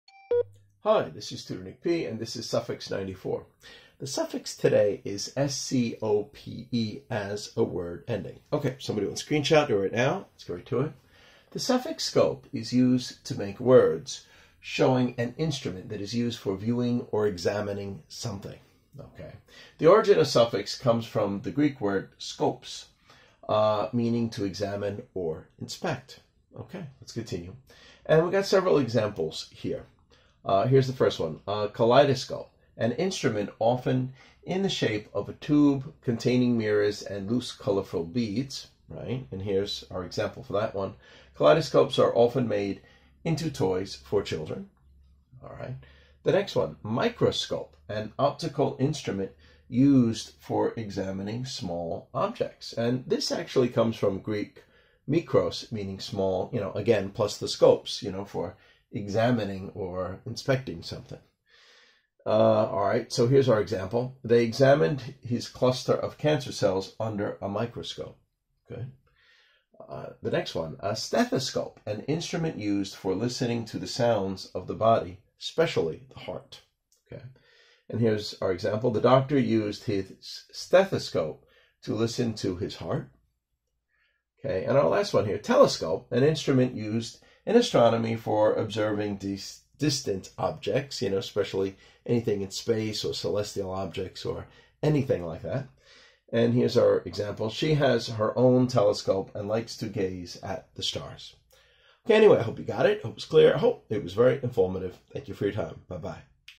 English Suffix (94) -Scope - Origin - English Tutor Nick P
In this video, we will cover the suffix -scope, allude to its origin, and give examples of its use. We will cover such terms as kaleidoscope, microscope, stethoscope, and telescope.